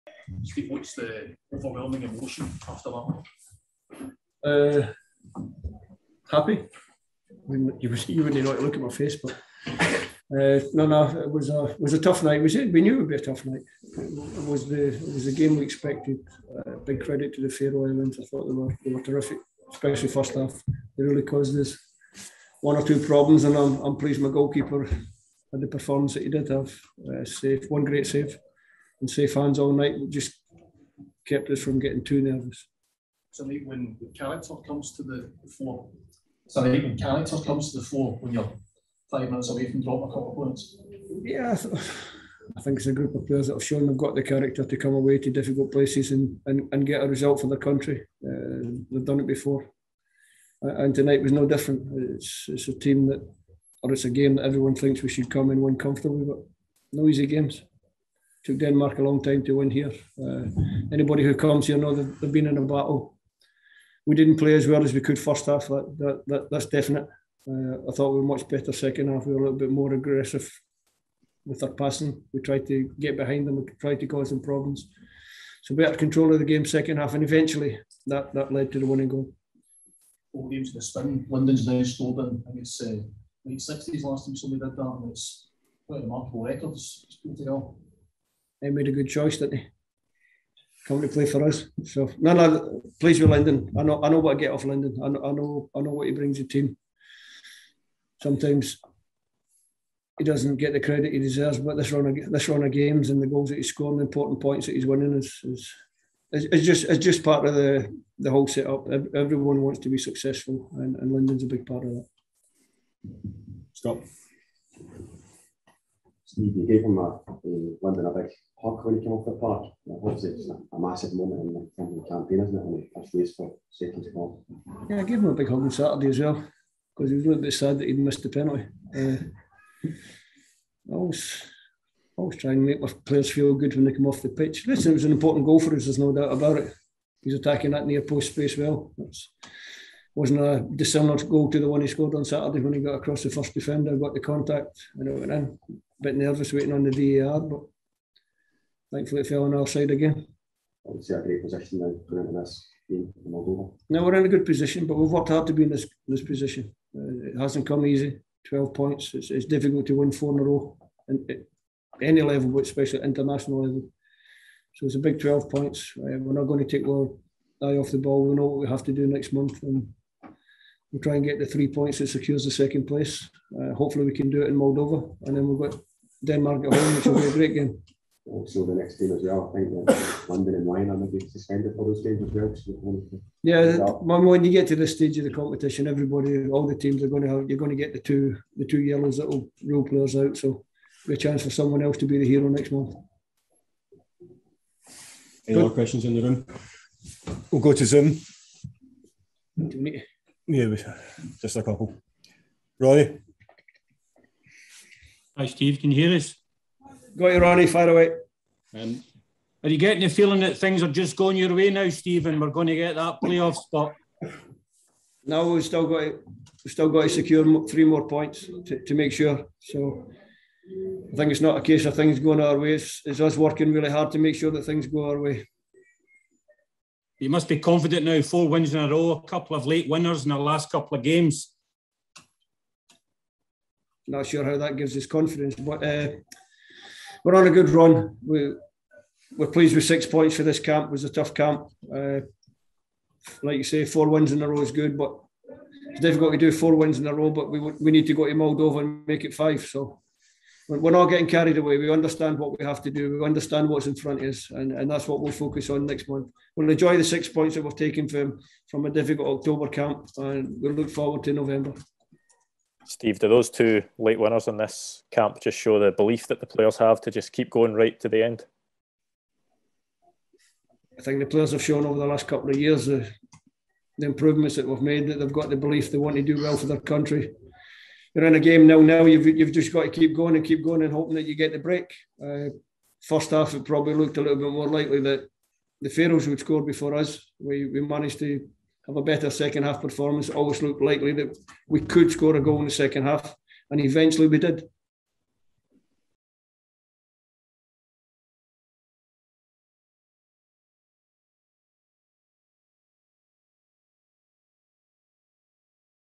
Scotland boss Steve Clarke thanks Lyndon Dykes for saving team from Faroe Islands humiliation
STEVE CLARKE thanked Lyndon Dykes for saving Scotland from another Faroes humiliation.

The big striker became the first player since Colin Stein in 1969 to score in four successive competitive games.

Dykes grabbed an 86-minute winner in Torshavn and Scotland can now seal a World Cup play-off place by beating Moldova next month.

Clarke said: "The record says Lyndon is in a good place and he's been a great addition to the squad since he chose to play for Scotland.

"We’re all grateful for that because he’s always committed and he score goals.

"It was a tough night and what we expected, to be honest.